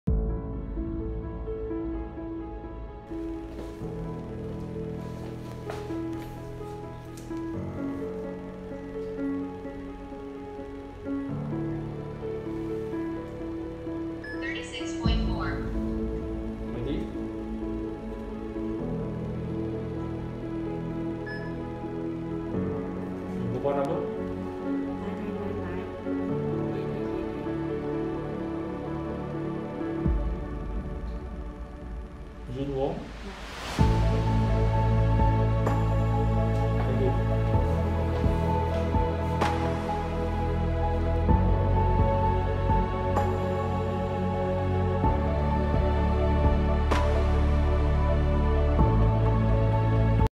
All-in-One Pocket Mobile Visitor Management System (VMS)
Leego all-in-1 Pocket Visitor Management System (Pocket VMS) is the lightest VMS that comes with temperature scanner, bar code/QR code scanner and VMS in a device.